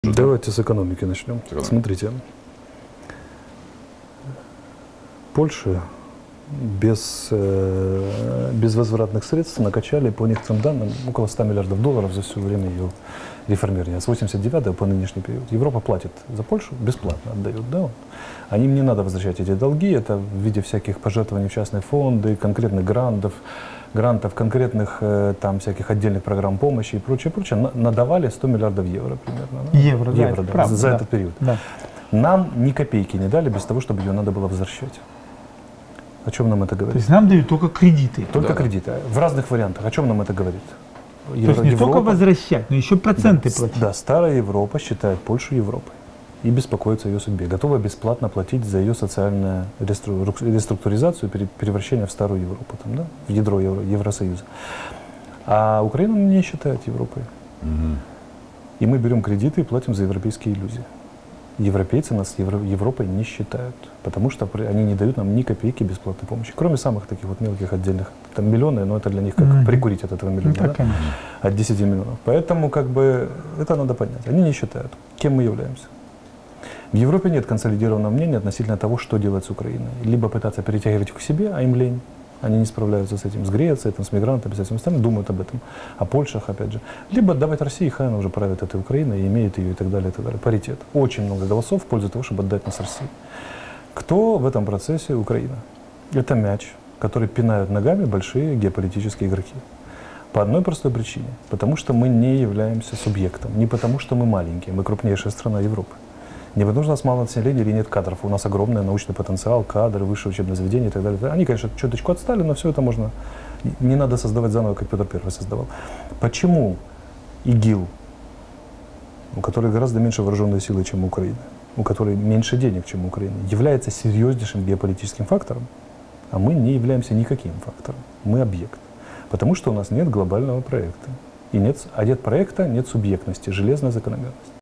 Европа не считает Украину Европой, - Алексей Арестович
Украина не является субъектом в мировых отношениях, в том числе и потому, что не имеет своего глобального  проекта.

Такую точку зрения в эфире UKRLIFE.TV высказал военный эксперт Алексей Арестович.

"В Европе нет консолидированного мнения относительно того, что делать с Украиной. Либо притягивать к себе, с чем Европа не справляется в силу внутренних проблем с Грецией, мигрантами и тому подобное, либо отдавать России", - сказал Арестович.

Полное интервью смотрите в эфире UKRLIFE.TV.